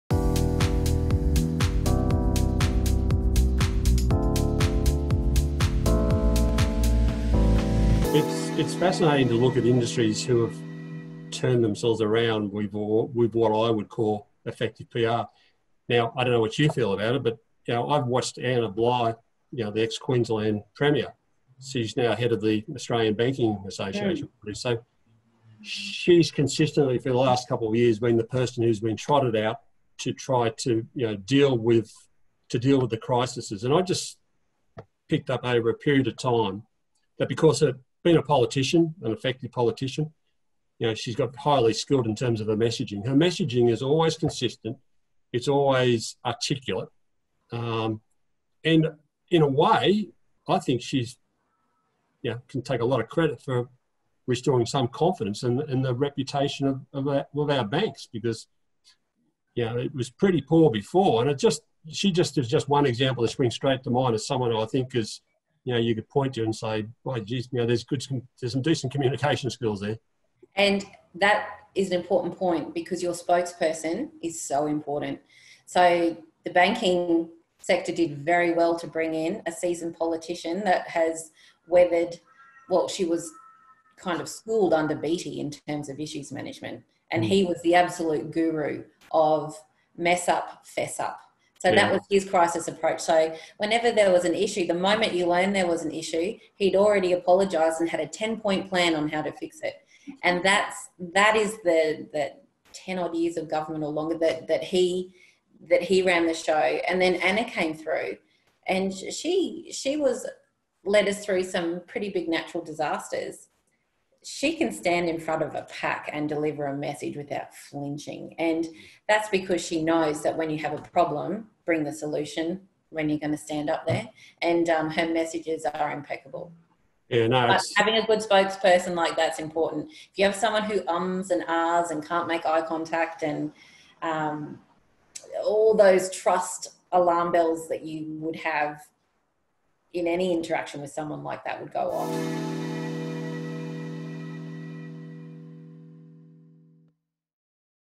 Helix Essential Series // Lessons from the banking sector with Sarah Dixon
Communication is Key with Sarah Dixon.

On 3 June 2020, Sarah Dixon from Urbaine Communications joined Michael Chesterman on the Helix Essential series to discuss communication strategy and issues management principles for your business as you navigate the COVID crisis and beyond. In this video, Michael and Sarah discuss lessons in dealing with crisis from the banking sector and the importance of a solid spokesperson